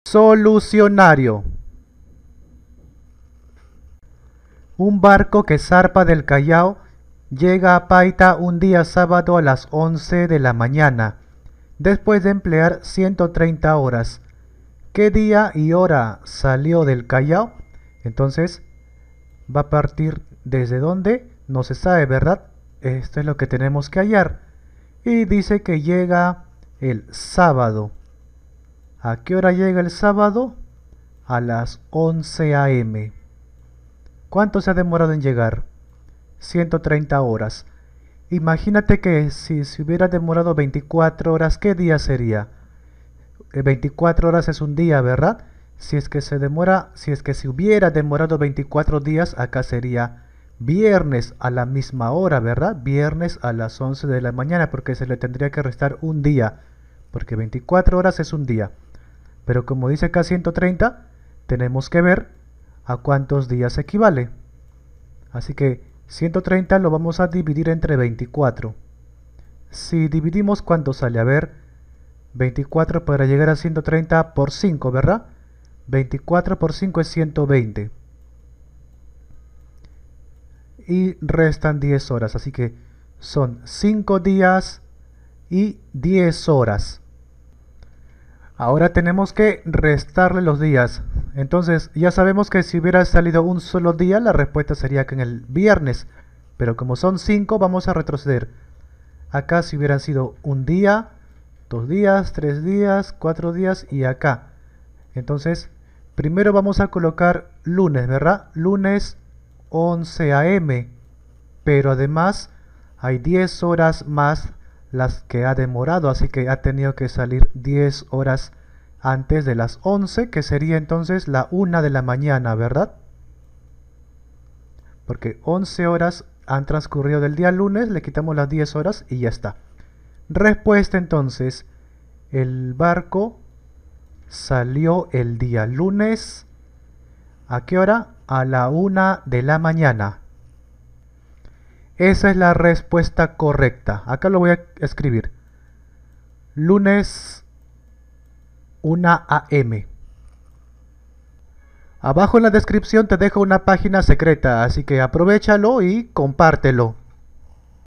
Un barco que zarpa del Callao, llega a Paita un día sábado a las 11 a.m después de emplear 130 horas
Un barco que zarpa del Callao, llega a Paita un día sábado a las 11 a.m., después de emplear 130 horas. ¿Qué día y hora salió del Callao?